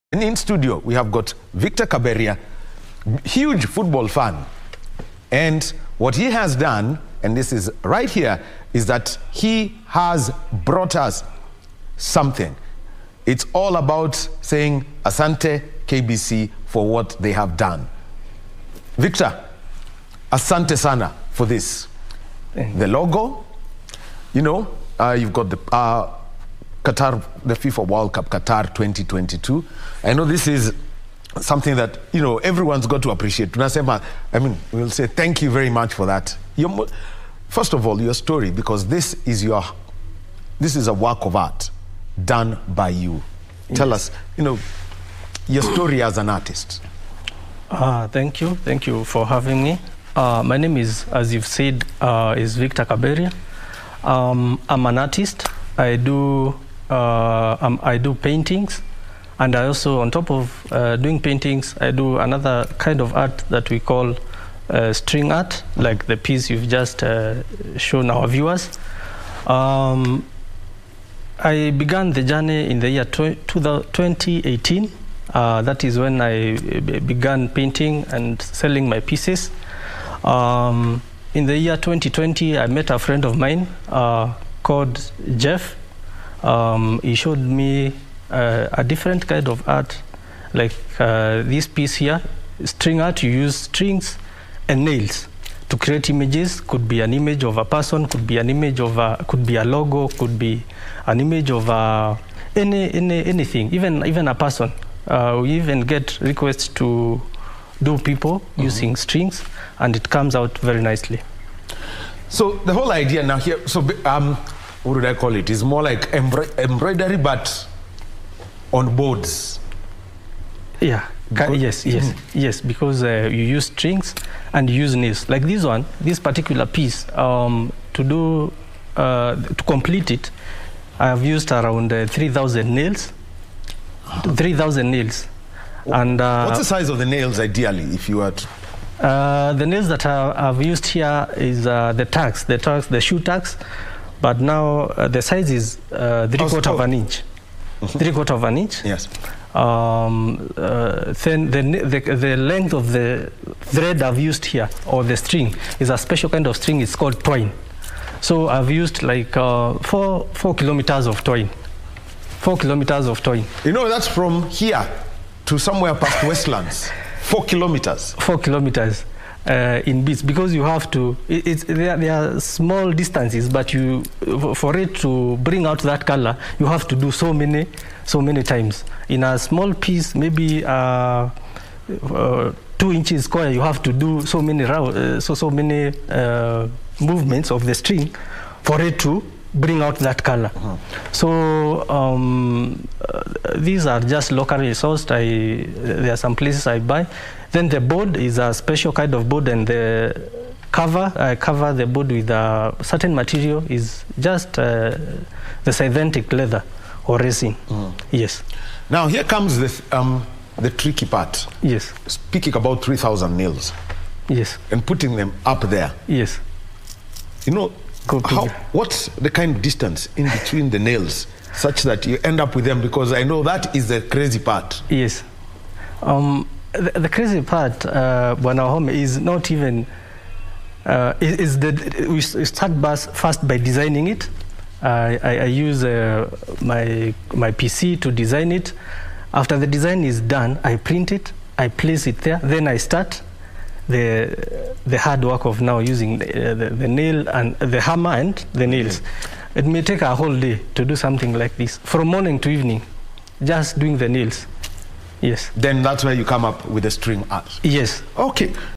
String Artist Victor Kaberia designs National Broadcaster's Logo with the FIFA 2022 World Cup logo
One On One with String Artist Victor Kaberia, who designed the National Broadcaster's Logo on a board with the FIFA 2022 World Cup logo.